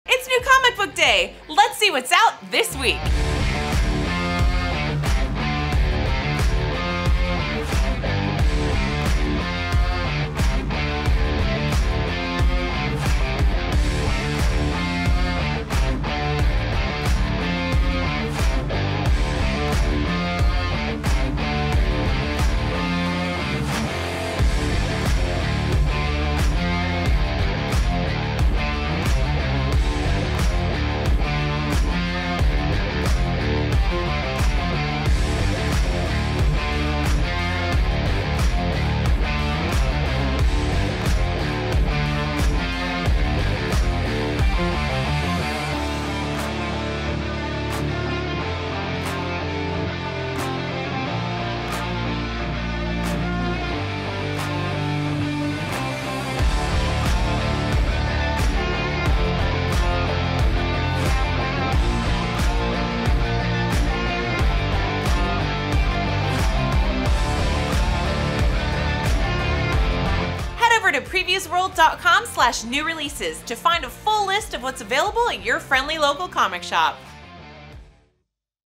WEB OF SPIDER-MAN, DUNE, COBRA COMMANDER, GARGOYLES: What's @ Comic Shops for 3/20/2024
WHAT'S @ COMIC SHOPS! Cannon Doll-X is here to remind you to check what's up and what's dropping at your LCS for the week of March 20th, 2024. Featuring Marvel, Dynamite, Dark Horse Comics, Image Comics, BOOM! Studios, VIZ Media, and more! See all of the incredible titles over

Host: Cannon Doll-X

Edited by Johnny Rose

Sapphire Studios